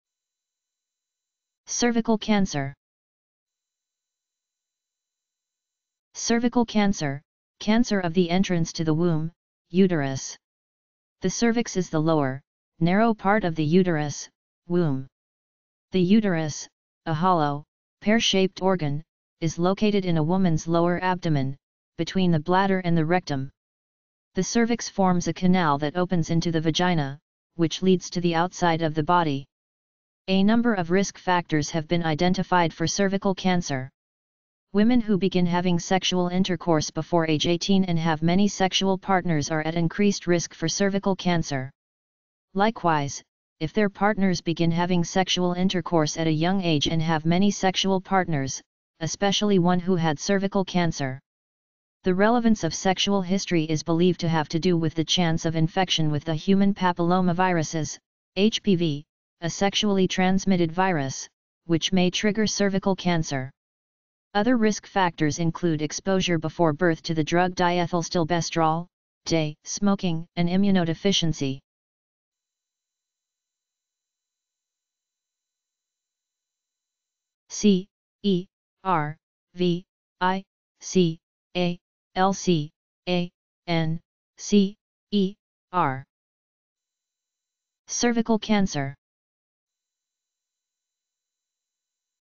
Cervical cancer - Medical Definition and Pronunciation
Title: Cervical cancer

Cervical cancer: Cancer of the entrance to the womb (uterus). The cervix is the lower, narrow part of the uterus (womb). The uterus, a hollow, pear-shaped organ, is located in a woman's lower abdomen, between the bladder and the rectum. The cervix forms a canal that opens into the vagina, which leads to the outside of the body. A number of risk factors have been identified for cervical cancer. Women who begin having sexual intercourse before age 18 and have many sexual partners are at increased risk for cervical cancer. Likewise, if their partners begin having sexual intercourse at a young age and have many sexual partners, especially one who had cervical cancer. The relevance of sexual history is believe to have to do with the chance of infection with the human papillomaviruses (HPV), a sexually transmitted virus, which may trigger cervical cancer. Other risk factors include exposure before birth to the drug diethylstilbestrol (DES), smoking, and immunodeficiency.